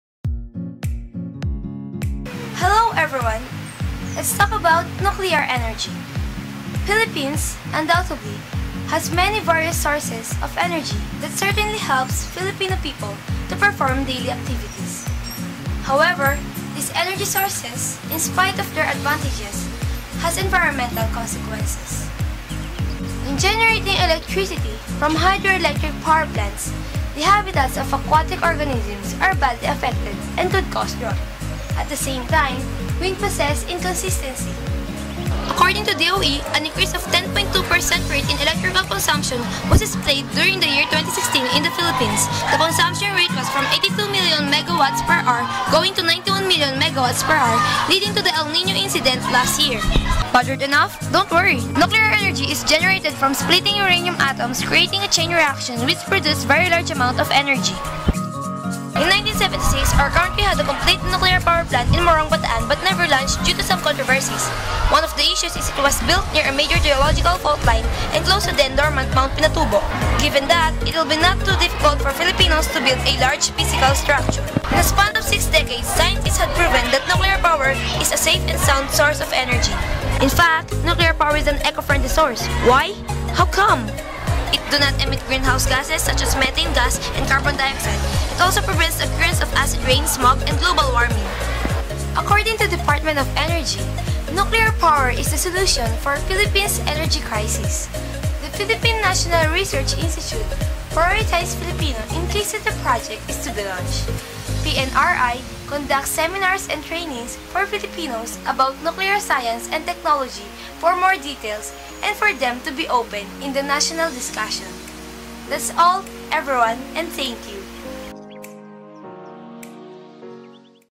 R4A-4: Bacoor National High School-Tabing Dagat Annex
"Nuclear Power for the People (Is Nuclear Power a Solution to Increasing Energy Demand of the Philippines?)"

Verbatim description of the video from the contestant:
Showcase of topic demonstration, brief and concise discussion and diverse animation effects. A combination of genuine and animation discussion is exhibited as the video presentation is being played. Nuclear science is the main content of the video presentation. It briefly unveils the answer to the question "Does the Philippines really need nuclear power?"